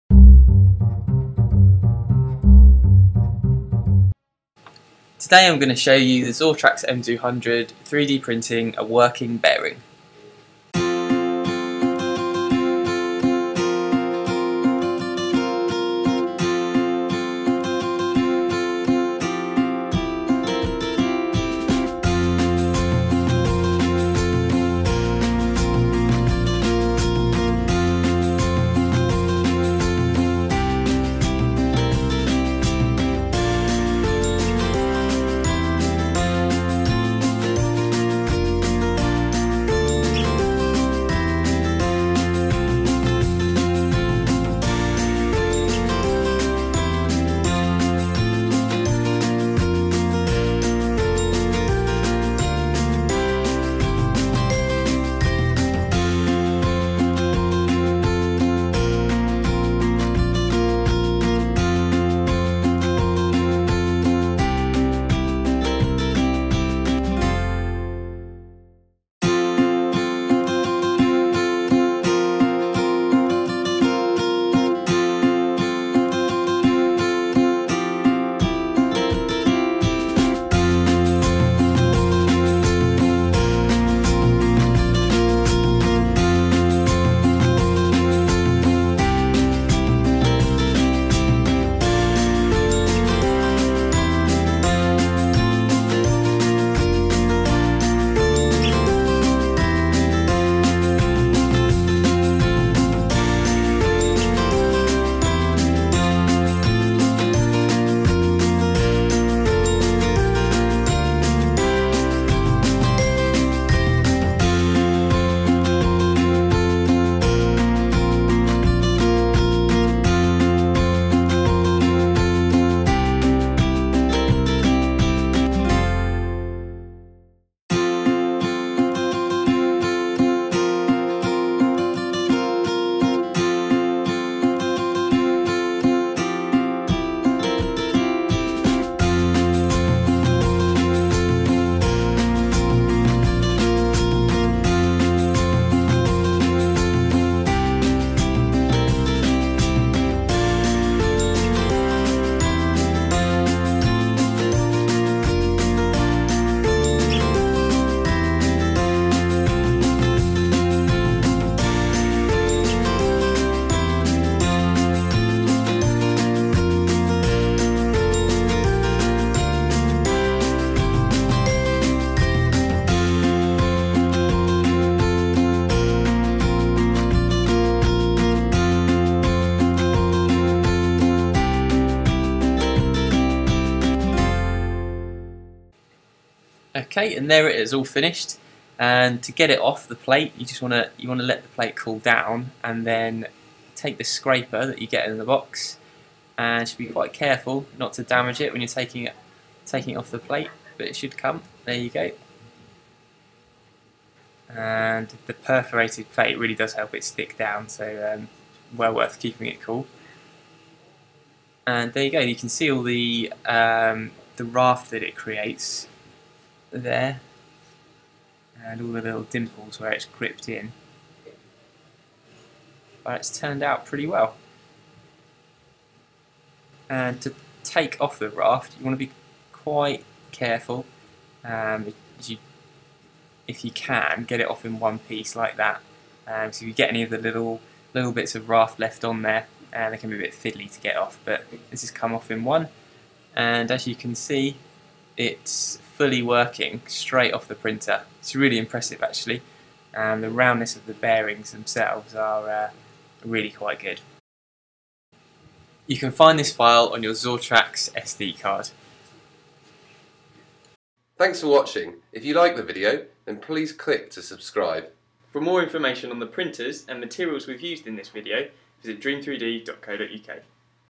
3D Printing a Working Bearing on The Zortrax M200 | Cool 3D Prints | Dream 3D
To find out more about this product, including price and availability please see the following

In this video I will be showing you the Zortrax M200 printing a working bearing. Footage speed is 20x faster.